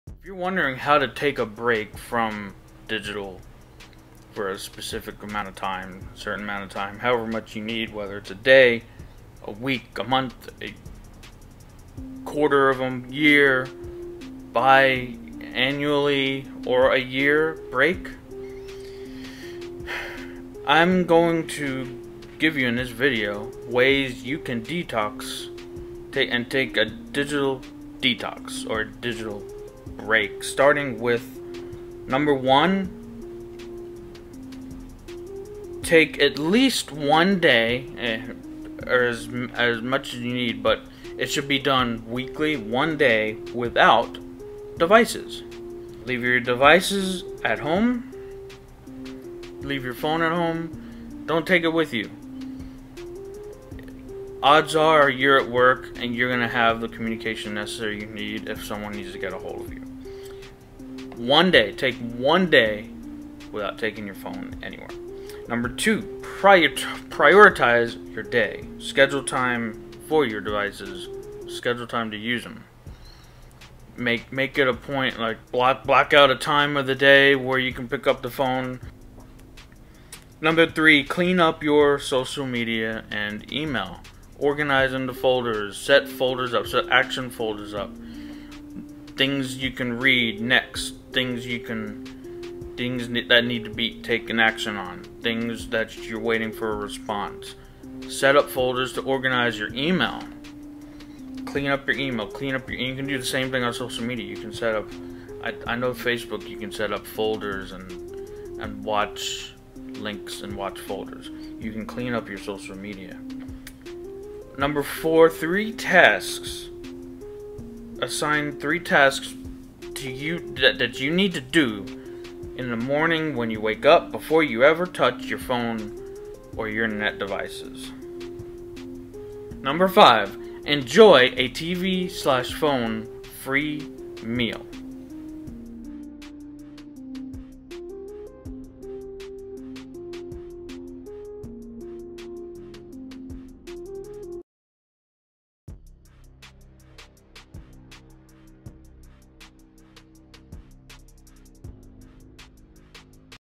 How to Clean Up Your Social Media Life | Social Media Detox Tips
I give my tips I use that helps me detox from social media so I am not feeling overwhelmed or overloaded.